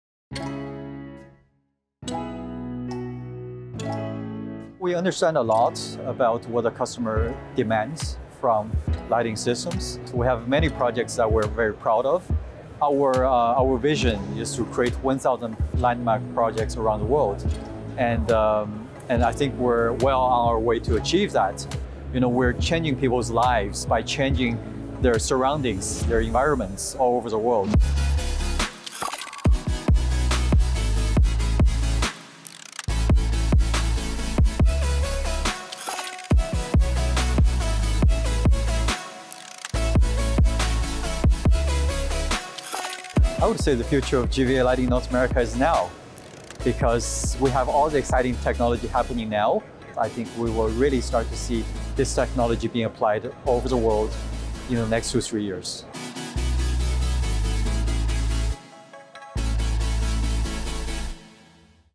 CNTV interview with John Zhu - Head of Sales (North America) at LightFair 2023
"We are changing people's lives by changing their surroundings." - John Zhu, Head of Sales, North America, GVA Lighting, Inc.

Enjoy this short interview video with John Zhu from Convention News Television, captured at LightFair in May 2023.